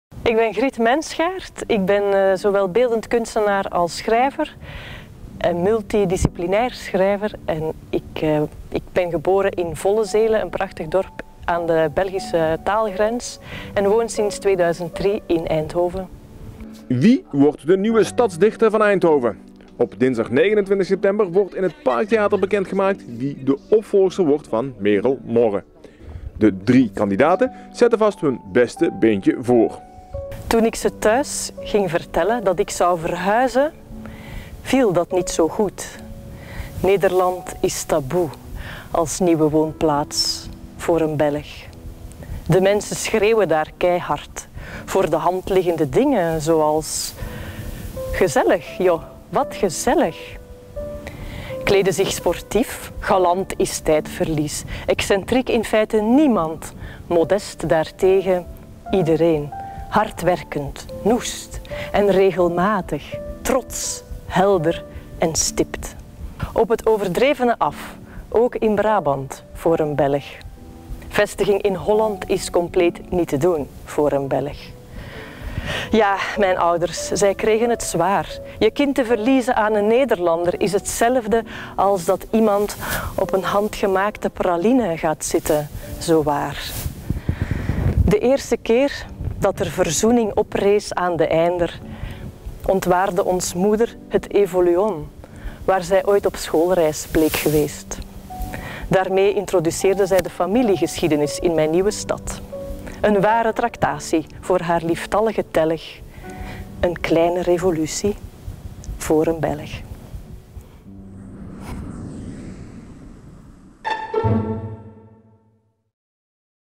Verkiezing Stadsdichter Eindhoven: Griet Menschaert
Wie wordt de nieuwe stadsdichter van Eindhoven? De drie kandidaten stellen zich voor. Finaliste nummer twee is Griet Menschaert.